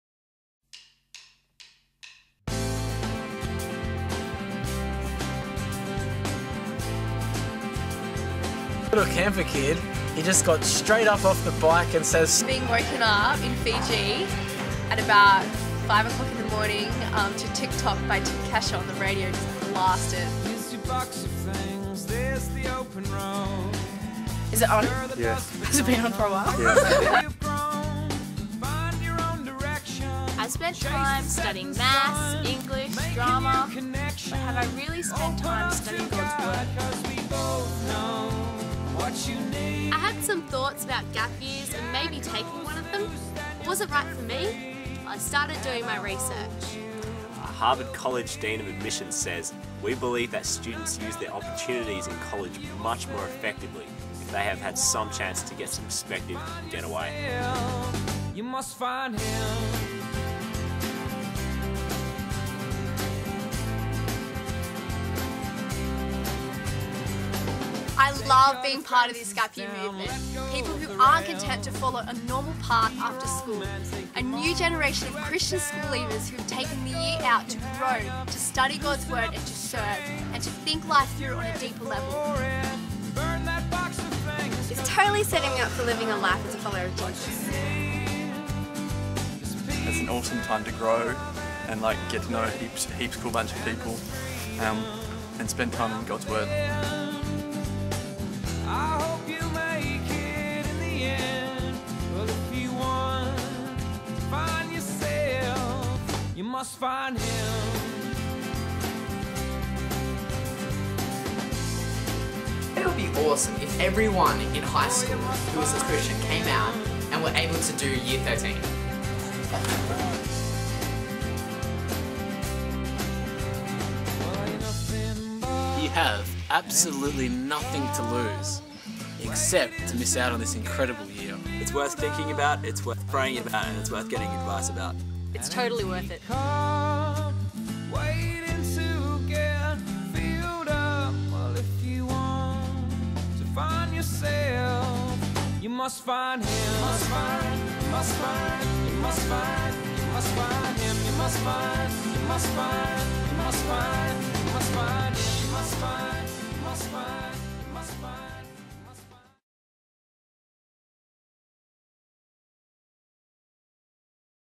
Year 13 Gap Year, 2011
Year 13 is a gap year program that runs out of Anglican Youthworks college in Loftus, Sydney.  We do a whole bunch of things but our main aim is that our students get to know Jesus better.